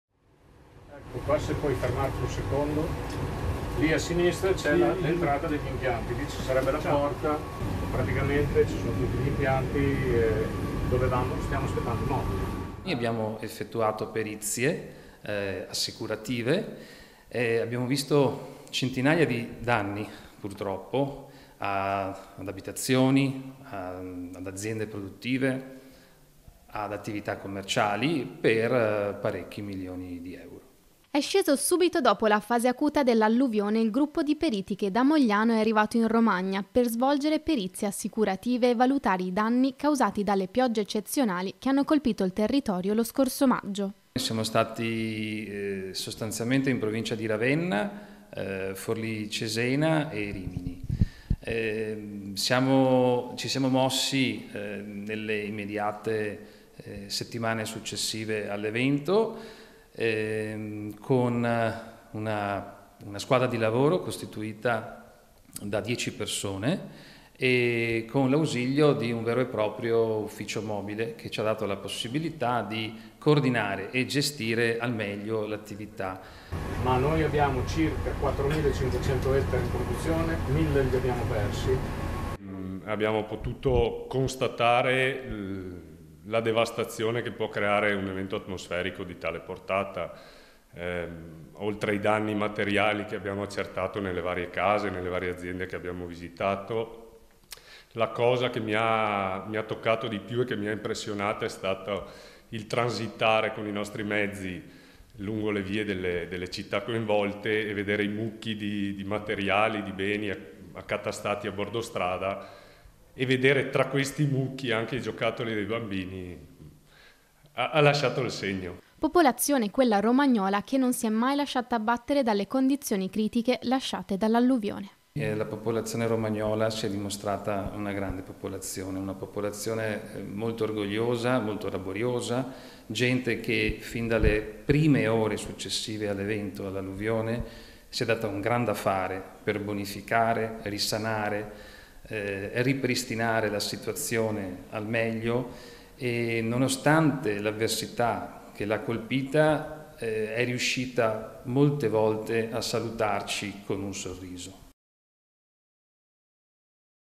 EMILIA ROMAGNA: IL VIAGGIO DEI PERITI NEL DISASTRO DELL'ALLUVIONE | 07/07/2023
A3 NEWS Treviso 07/07/2023 - MOGLIANO VENETO - Non solo Protezione Civile e Vigili del Fuoco. Nell'Emilia Romagna devastata dall'alluvione ha lavorato in queste settimane anche una squadra di esperti di Mogliano Veneto per svolgere perizie assicurative. Situazione critica: milioni di euro di danni - Intervistati ALESSANDRO GAVELLI (Presidente e Amministratore Delegato AG), GIANLUCA SOLAGNA (Responsabile operation AG ) (Servizio di Edy Caliman) - Segui Antenna Tre anche sul digitale terrestre! Visita il sito